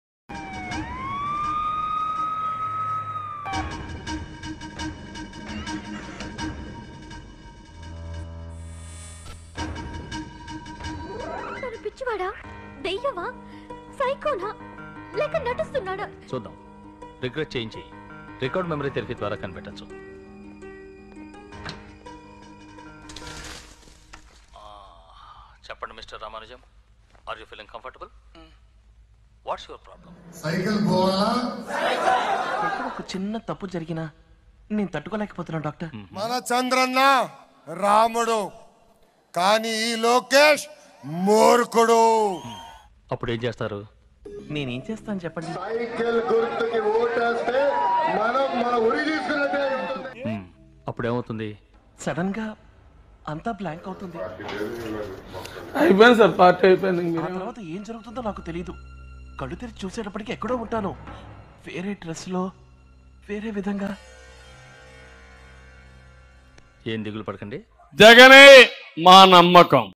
New Drama of Nara Lokesh to Avoid Padayatra | నారా లోకేష్ కొత్త డ్రామా | YuvaGalam | Funny Troll
అపరిచితుడుగా మారిన పప్పు లోకేష్ !! ఎండలో పాదయాత్ర చెయ్యలేక కొత్త ప్లాన్.
New Drama of Nara Lokesh to Avoid Padayatra | Nara Lokesh Funny Troll